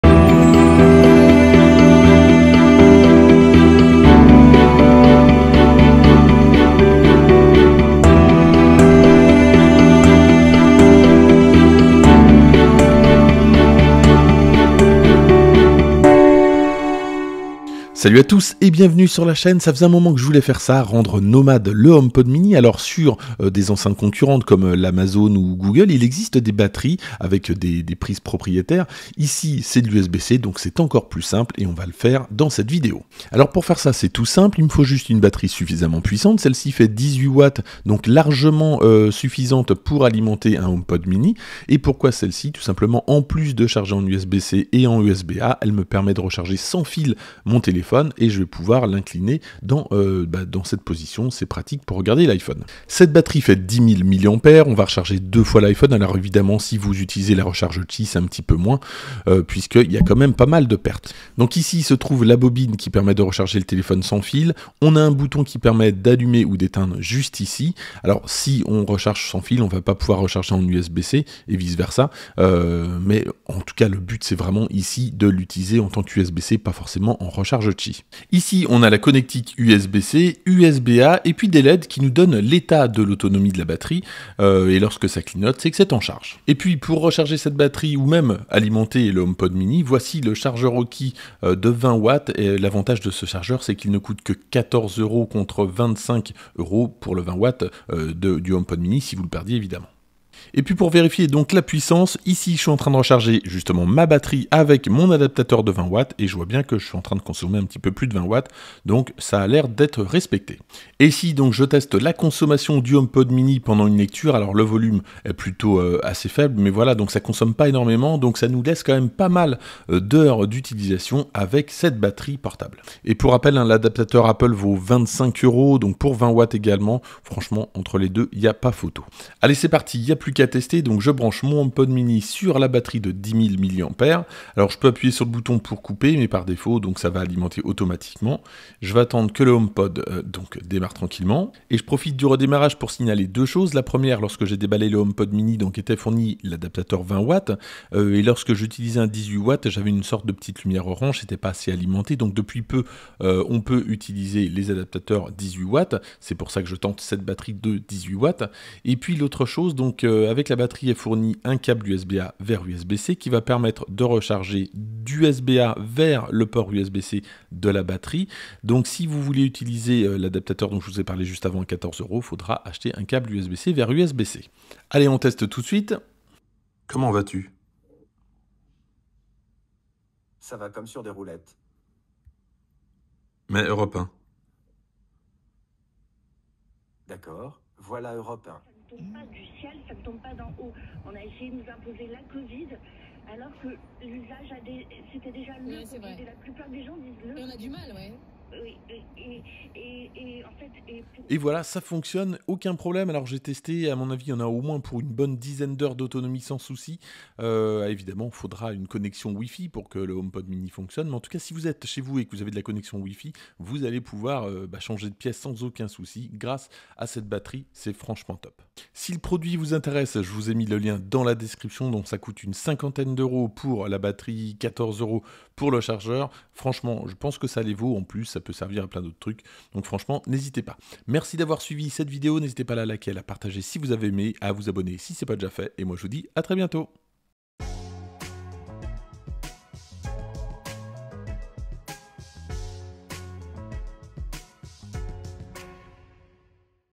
HomePod mini : Comment le rendre portable avec un powerbank
Dans cette vidéo, je teste le Powerbank 10000 mAh de la marque AUKEY pour alimenter mon HomePod mini. Je teste également le nouveau chargeur secteur de 20W à 14 euros !

🔋Le PowerBank AUKEY 10000 mAh
🔌Adaptateur secteur AUKEY 20W USB-C